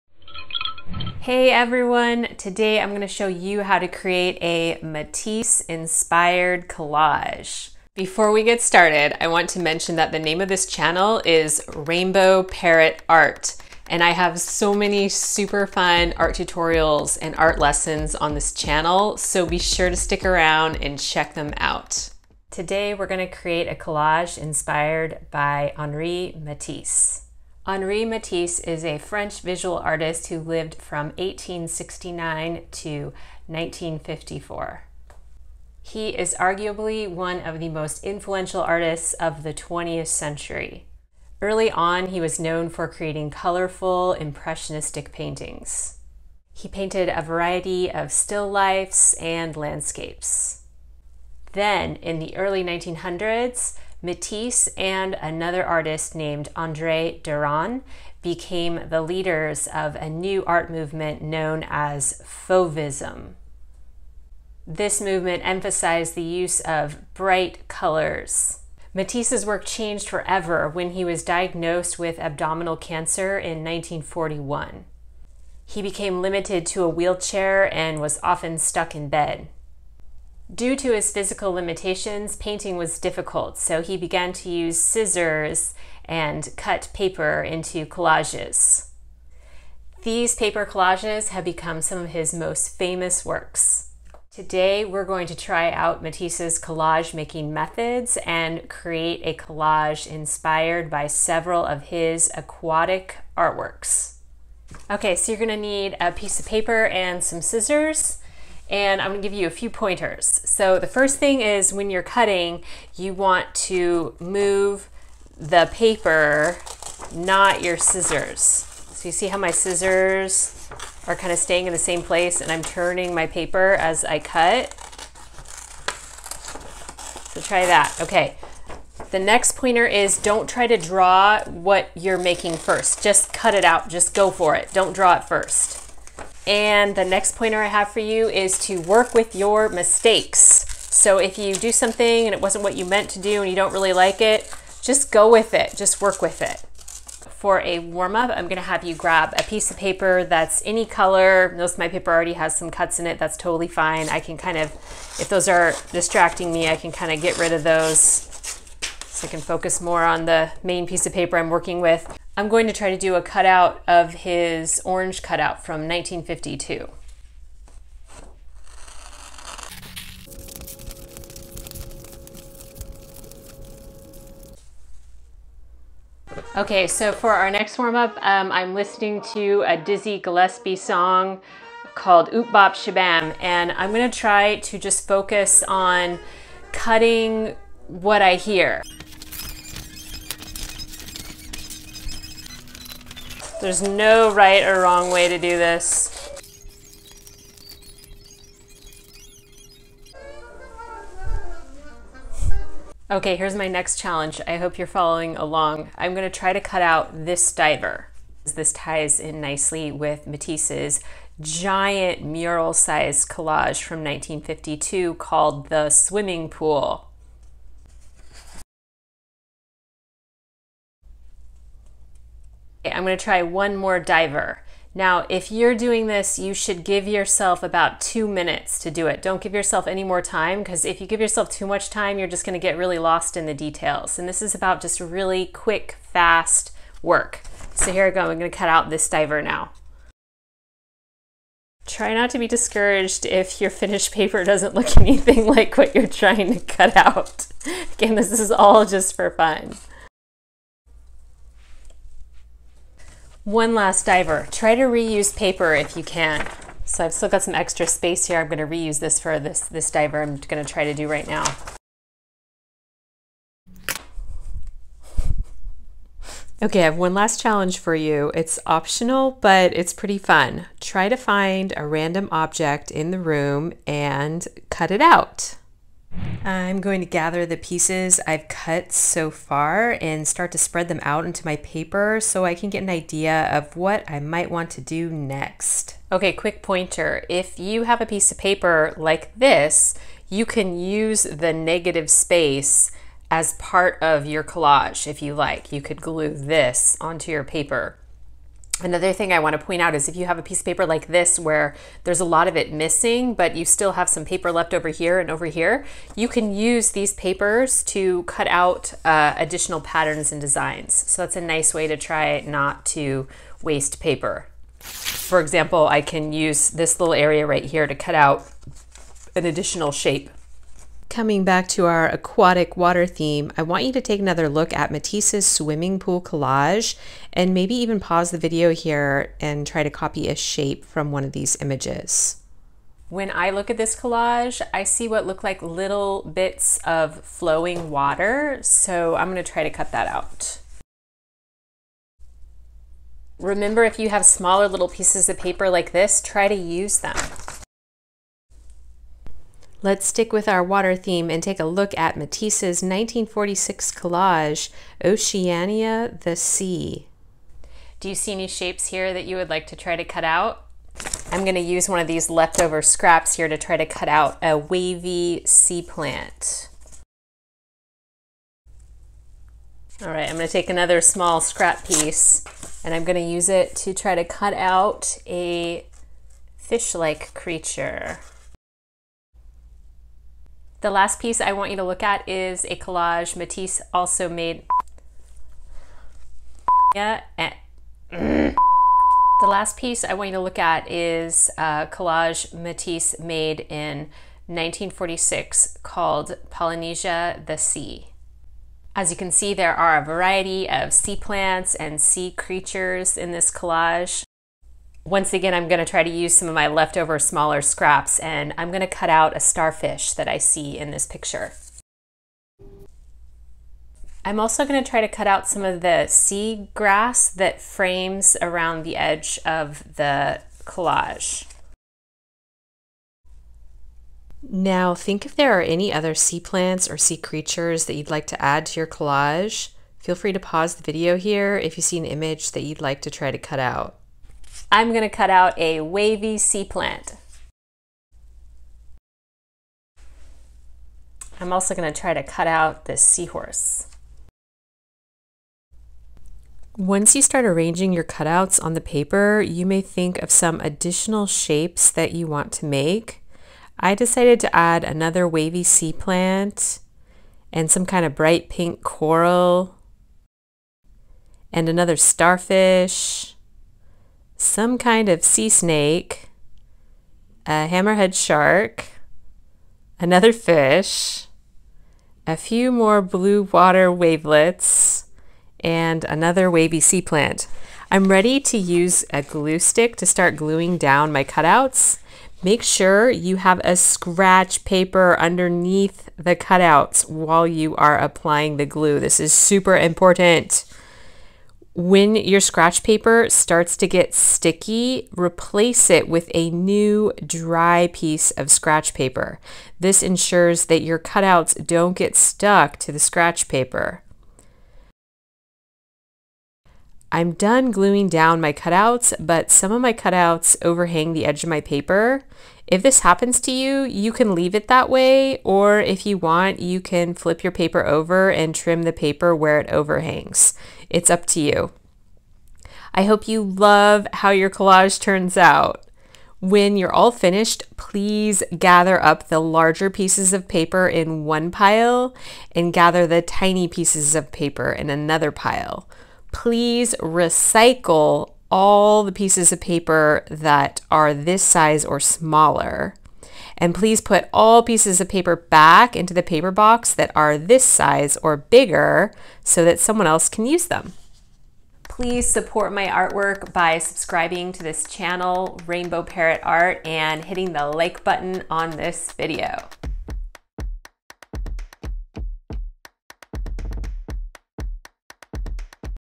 Master Matisse's Genius Collage Techniques | Step-by-Step Art Tutorial
Discover the art of collage with this easy, step-by-step Matisse-inspired project! Perfect for kids, teachers, and parents, this tutorial shows you how to create your own 'Painting with Scissors' masterpiece using paper cut-outs, just like Henri Matisse. Whether you're a beginner or an experienced artist, this fun and educational art lesson is ideal for elementary and middle school students. Dive into the world of 'découpage' and make a colorful collage that’s perfect for all ages and skill levels!

👉You'll love this collage project!
✂️ Amazing Collage Technique with Cereal Boxes | Step by Step